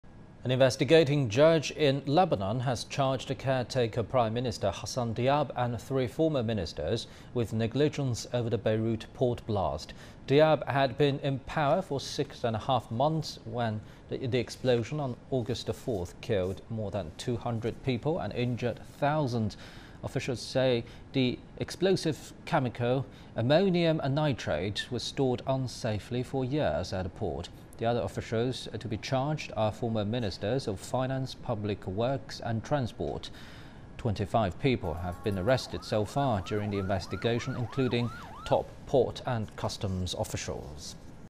Judge charges PM, 3 ex-ministers over Beirut port blast
An investigating judge in Lebanon has charged Prime Minister Hassan Diab and three former ministers with negligence over the Beirut port blast.